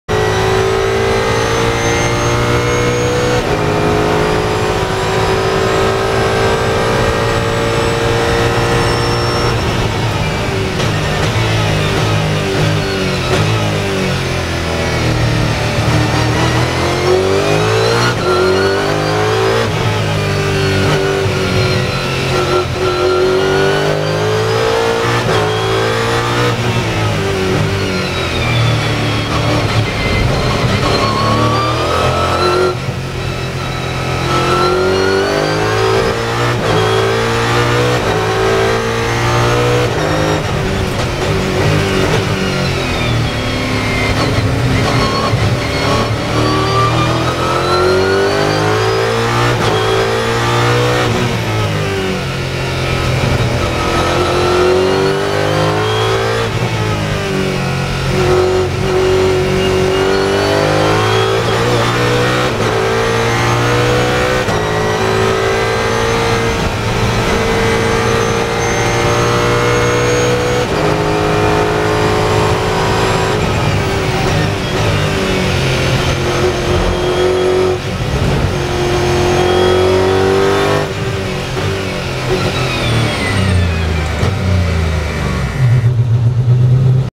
LEITSPEED: VBOX HD Alzen Ford GT Training VLN3 Freitagsession
Inlap des Ford GT von Jürgen Alzen Motorsport beim Training zu VLN3 2014.
Onboardvideo und Datenaufzecihnung mit VBOX HD: 720p HD Kamera mit Global Shutter für stabilstes Bild im Motorsport und 10 Hz GPS Datenlogger.